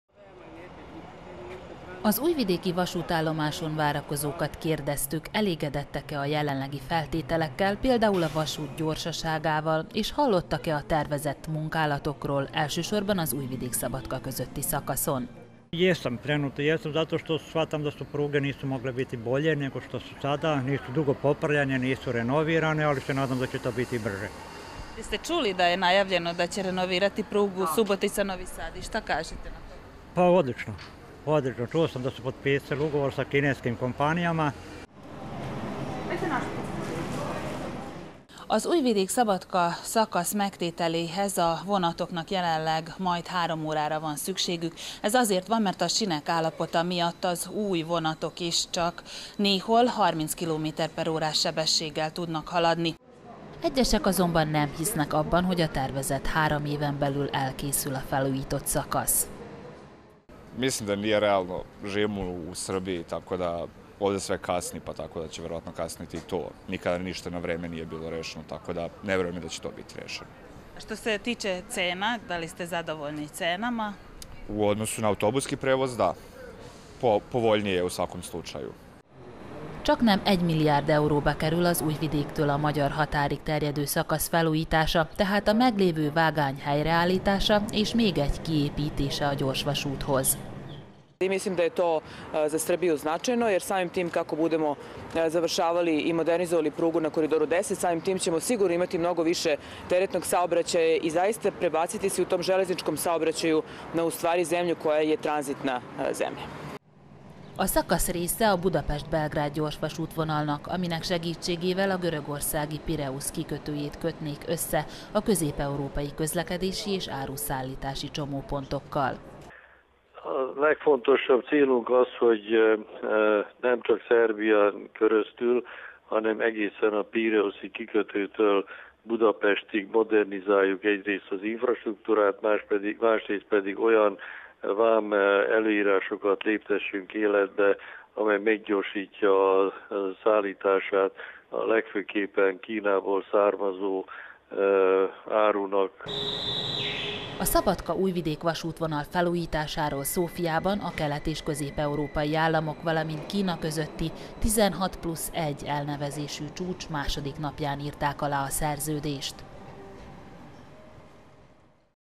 2018.07.10. - Hamarosan felgyorsulhat a vasúti közlekedés Szerbiában
Ha vonattal utazunk, lassabban jutunk el egyik helyről a másikra, viszont így jóval olcsóbb az utazás, mint autóbusszal.
A bejelentések szerint ez már három év múlva változni fog, a 10-es közlekedési folyosó vasútvonalán végzendő felújítási és építési munkálatoknak köszönhetően.